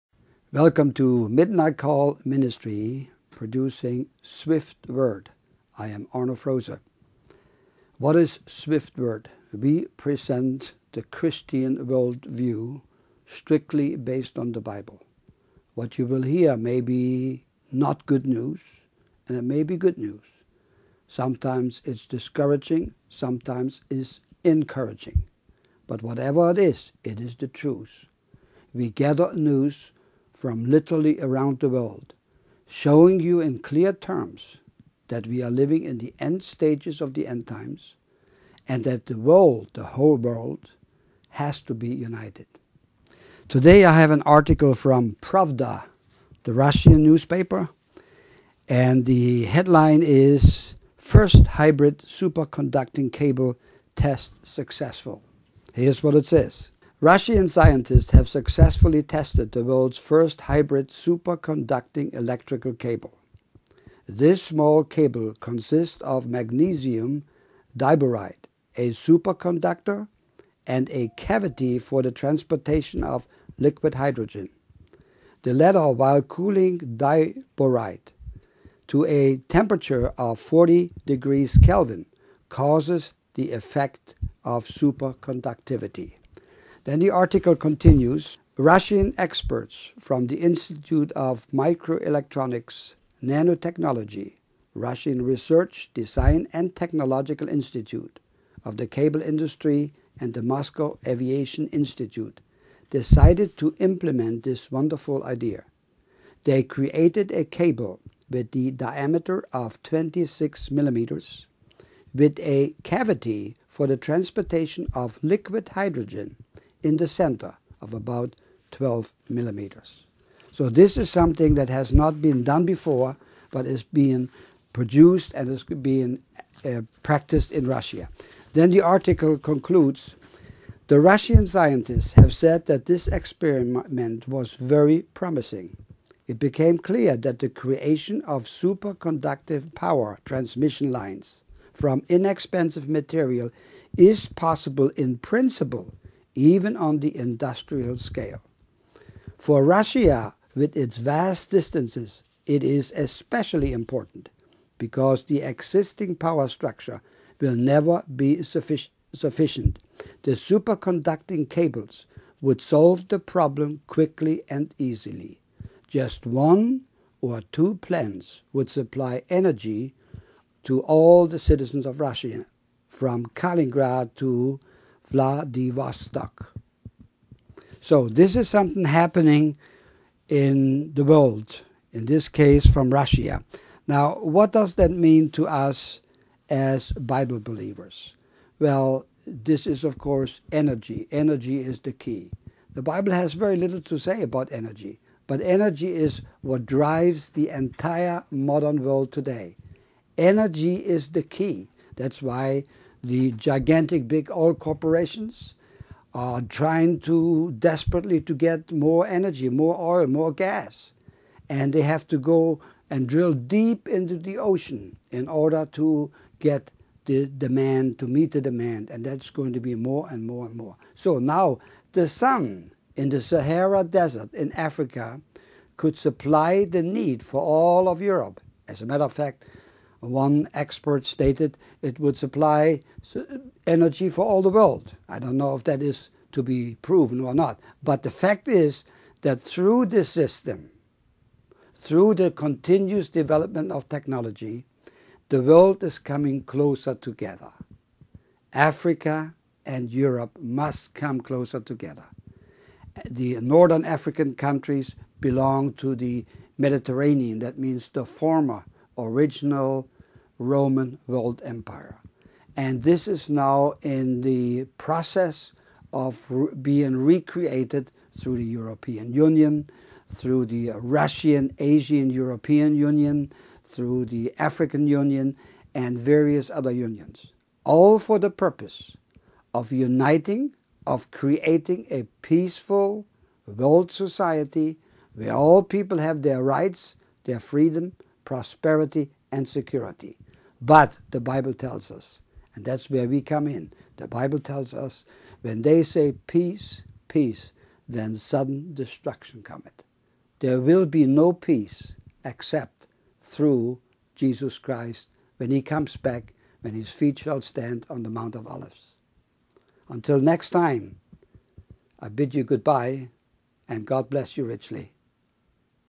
Swift Word #3 - Russia's Superconducting Power Cable
"World's first hybrid superconducting electrical cable tests successful" and further global energy ties between Europe, Africa, and Asia.